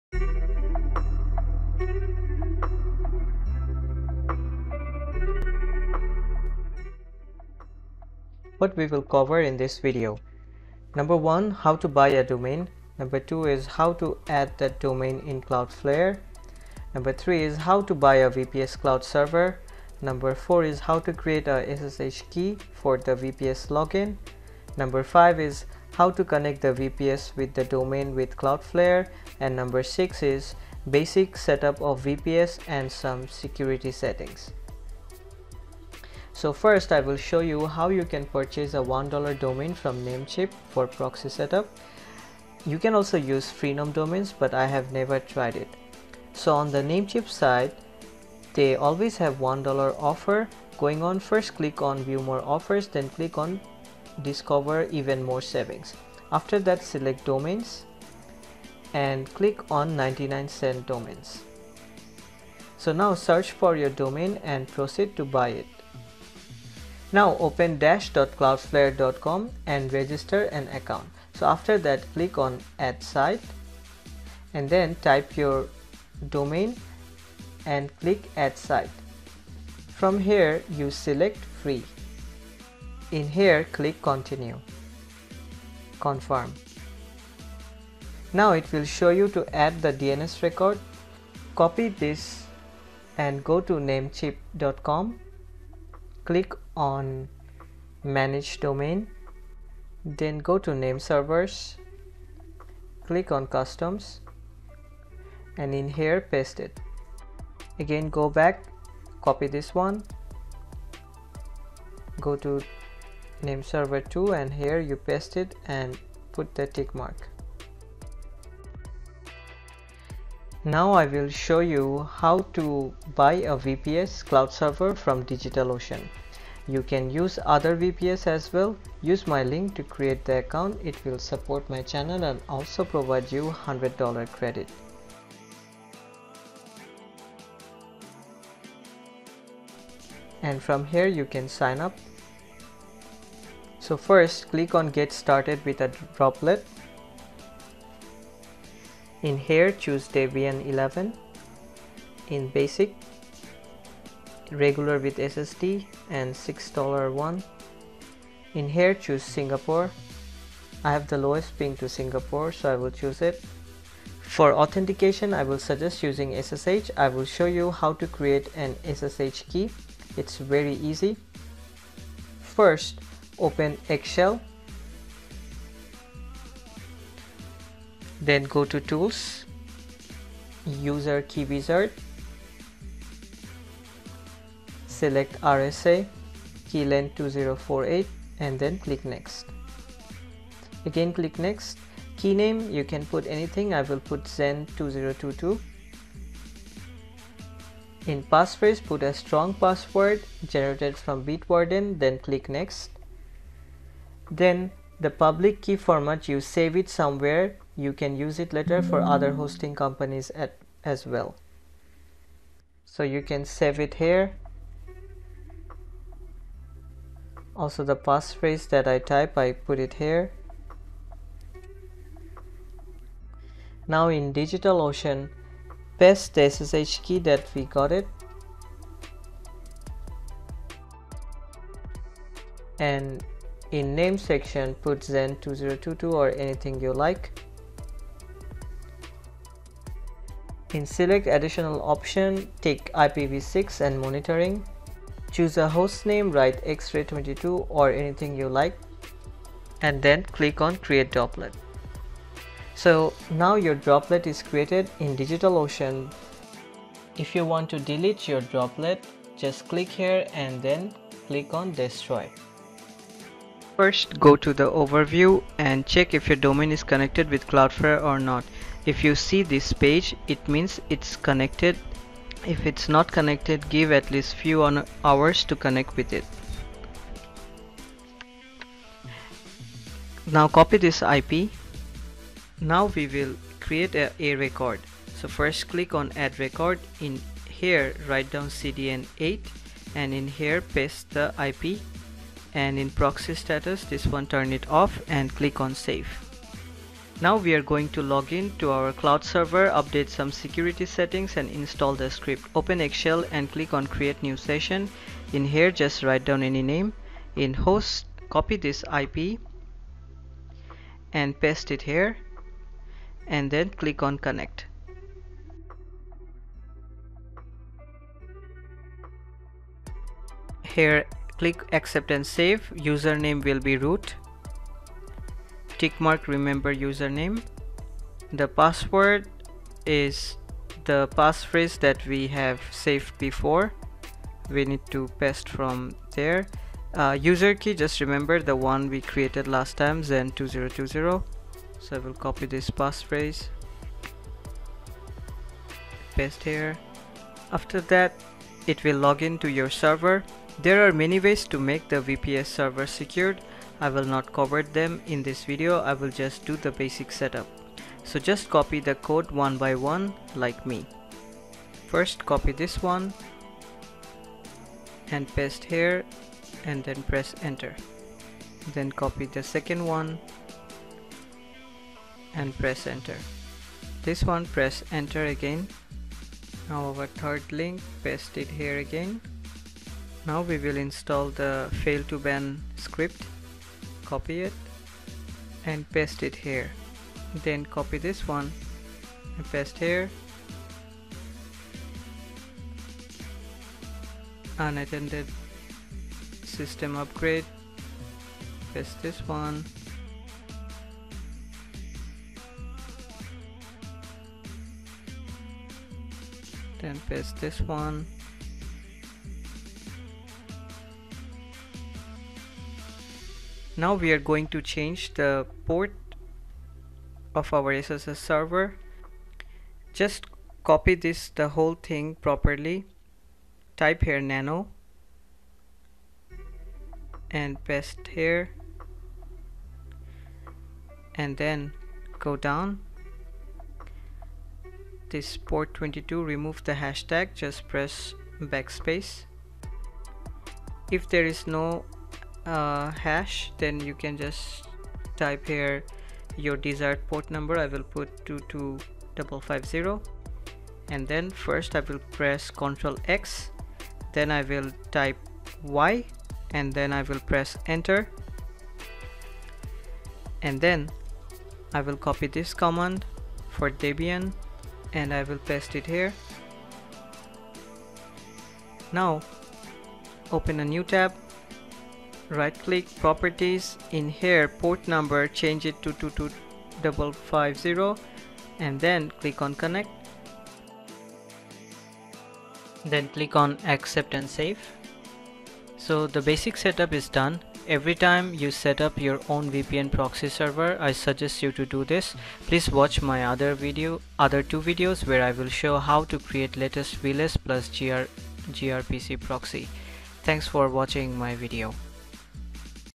Things you need to prepare before installing Proxy / VPN - Proxy Series - Part 1 - Xray | v2ray
What we will cover in this video:
0:00 - Intro
0:36 How to buy a Domain
1:06 How to add that domain name in Cloudflare.
1:59 How to buy a VPS/Cloud Server.
2:45 How to create a SSH Key for VPS Login.
4:14 How to connect the VPS with the domain via CloudFlare.
4:58 Login to VPS server with xShell.
5:48 Basic setup of VPS and some security settings.

You can find all the scripts that you have to copy paste

In my previous videos, I have never shown you the basic configuration, and many people keep asking me some basic questions, so I created this video. It is helpful for beginners.